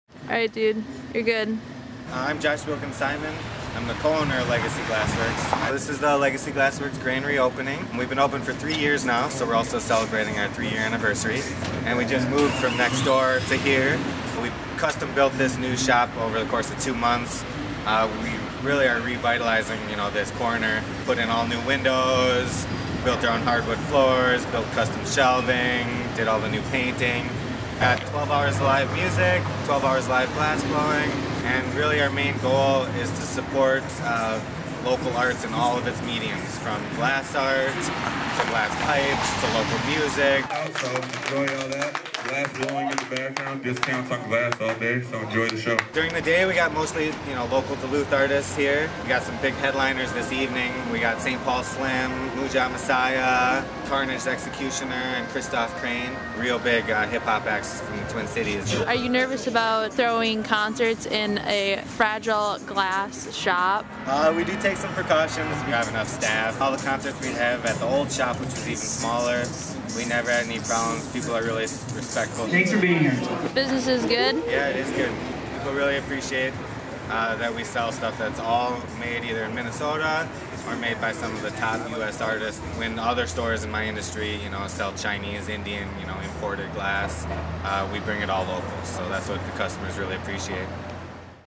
Legacy Glassworks re-opening
Duluth, Minn.-- The tobacco shop/art gallery/music venue, Legacy Glassworks, ditched the cramped storefront and moved down, right around the corner to a more spacious unit still on 1st Street West. Co-owner Josh Wilken-Simon told Lake Voice News writer, Kim Hyatt, the much needed space can be used to house a variety of artists from glass blowing to DJing. The grand re-opening to celebrate the expansion on Saturday, Oct.19 featured local artists from Duluth, Minneapolis and all around the Midwest.

Photojournalist: Kim Hyatt
Editor: Nate Kruse